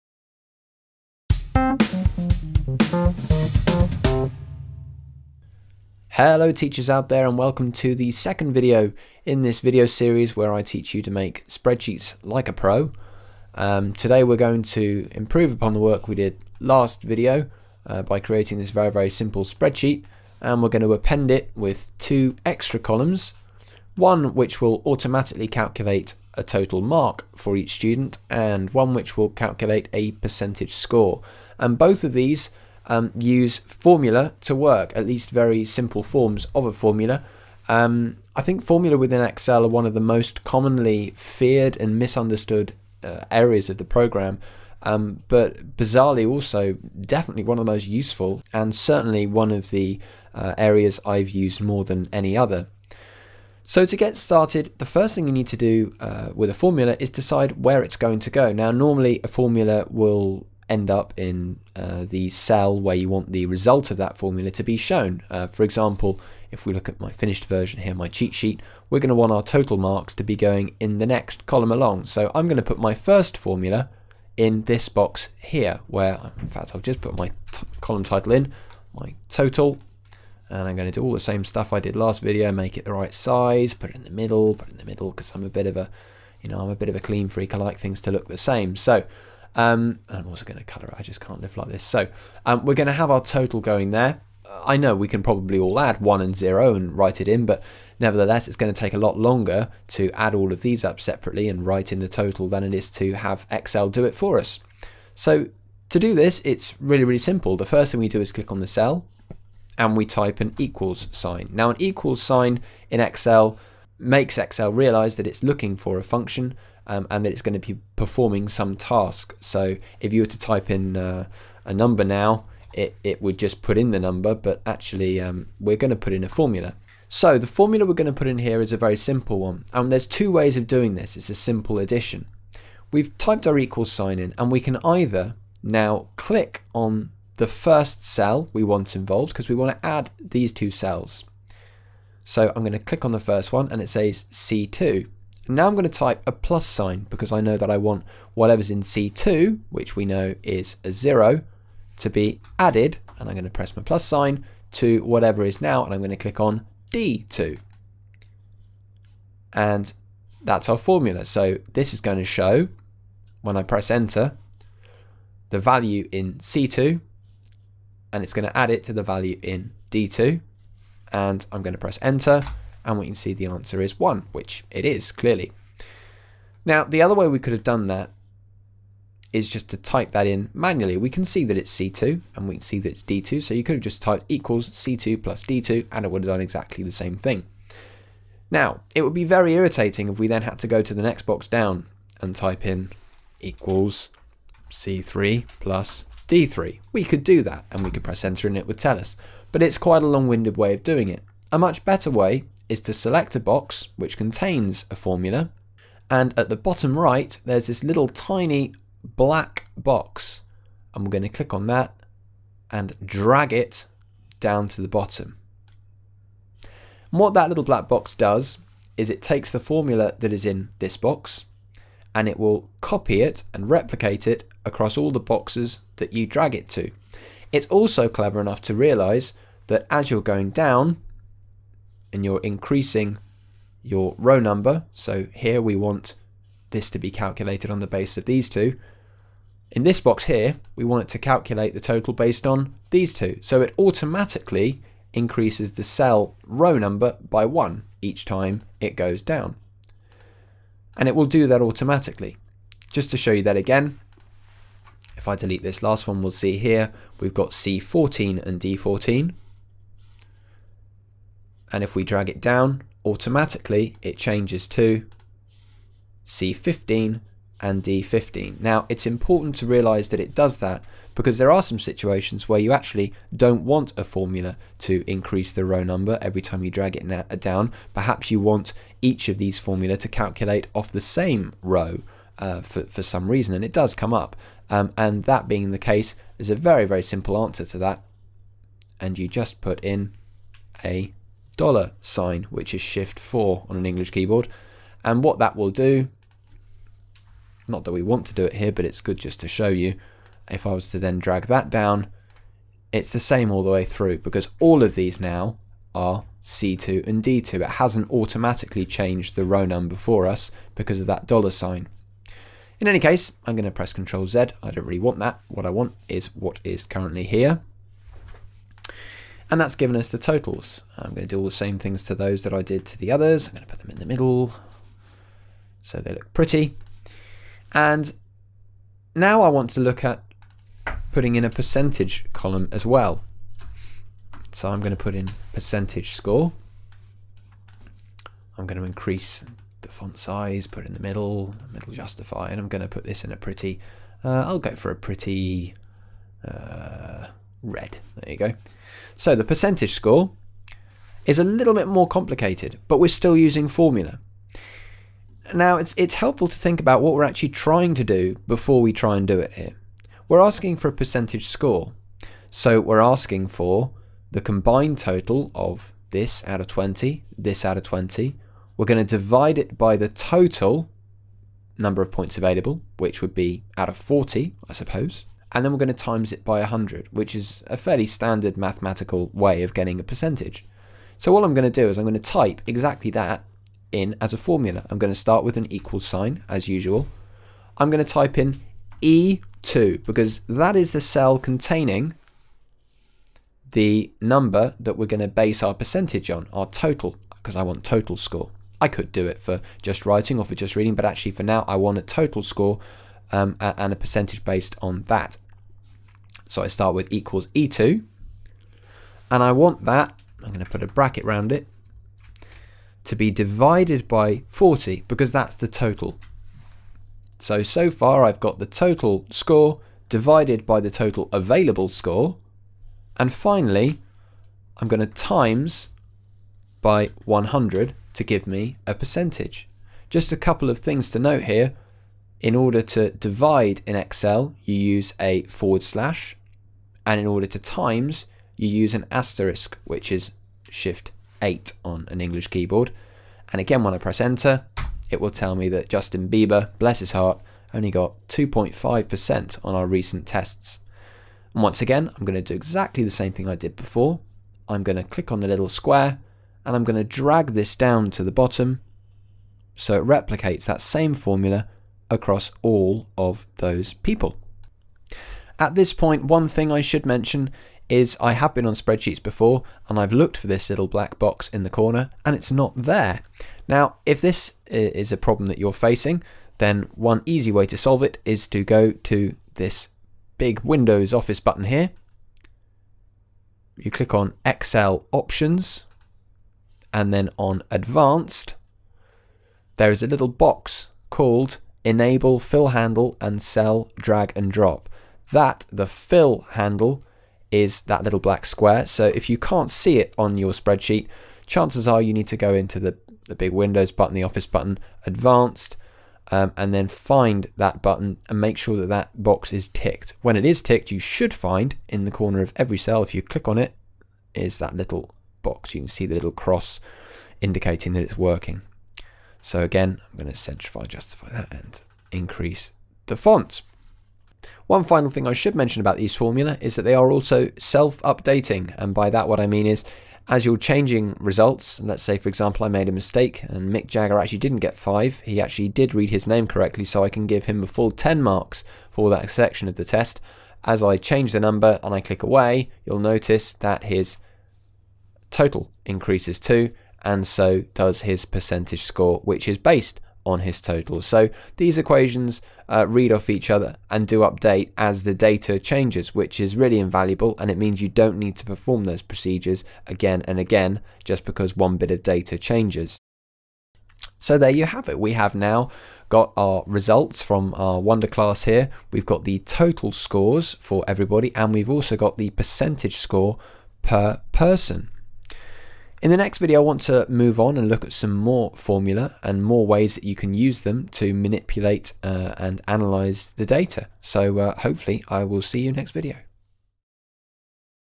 Excel Tips for Teachers Episode 2: Using Basic Formulae
In this video series I show you some of the handy excel tips to get you creating and using your own spreadsheets. In this video, we look at how to use basic formulae to perform simple tasks in Excel such as automatically adding the values in two cells, and calculating percentage scores.